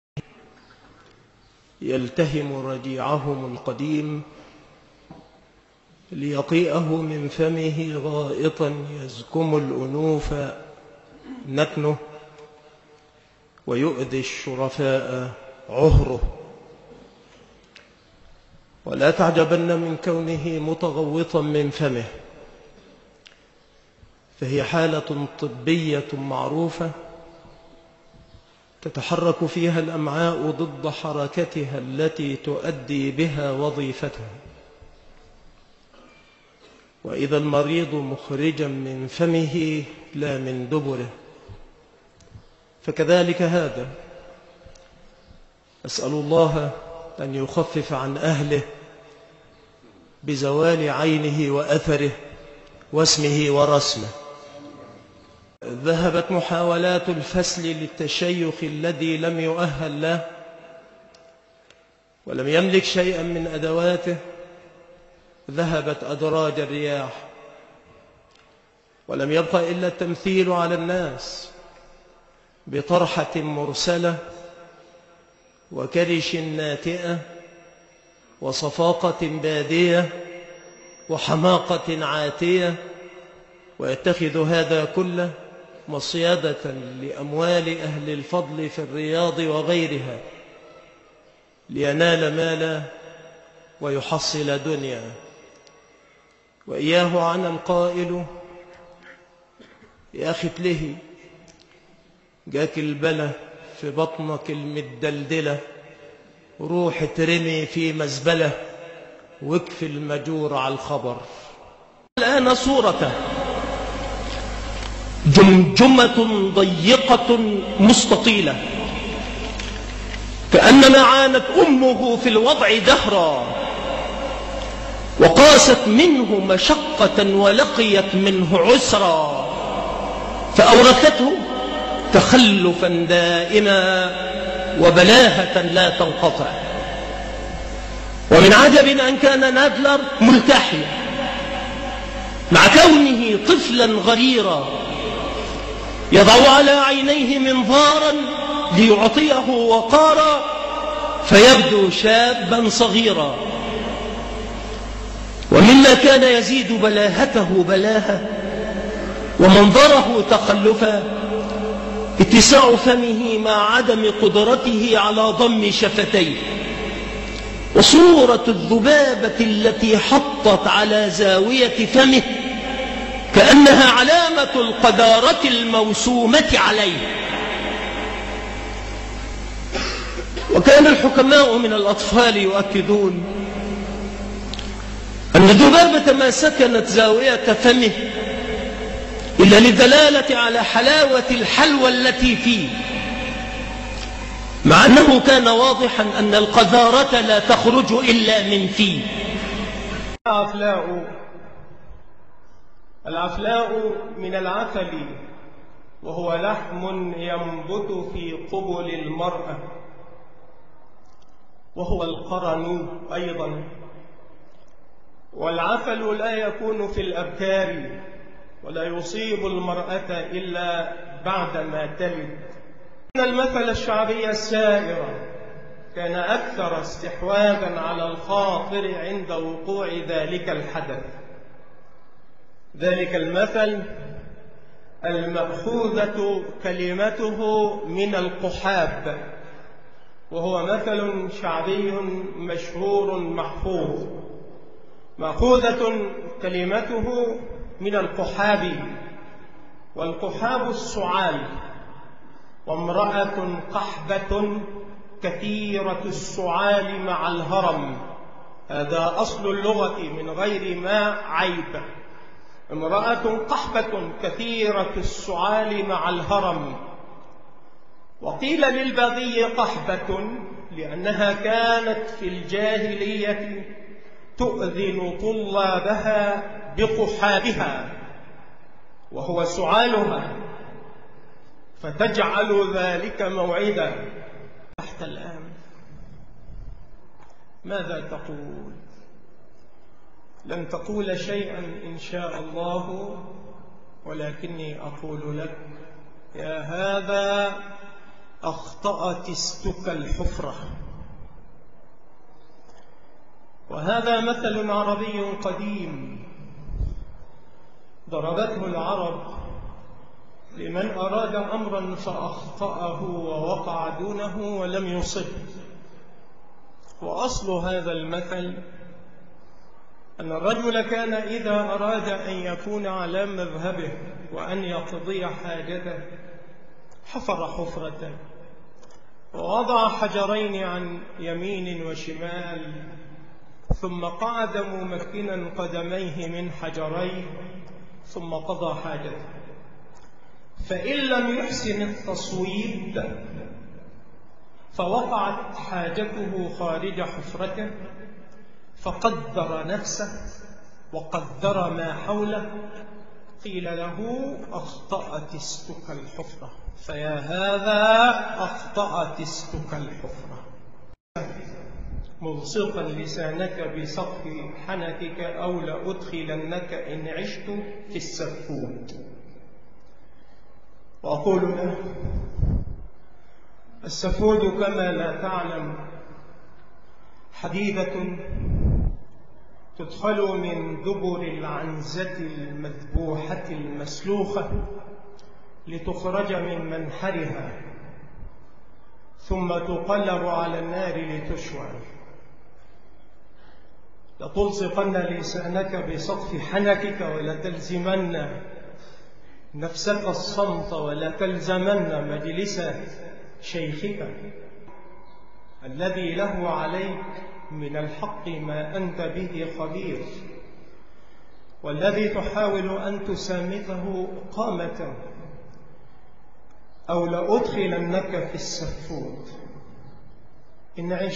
المنافق رسلان وفجوره في الخصومة
Мой НОВЫЙ канал в телеграм تولى المشركين والمرتدين فطبع الله على قلبه وأوقعه في النفاق والفجور قاتله الله Подписывайтесь на мой канал в Телеграм, там много переводов и текстовых записей, которых нет тут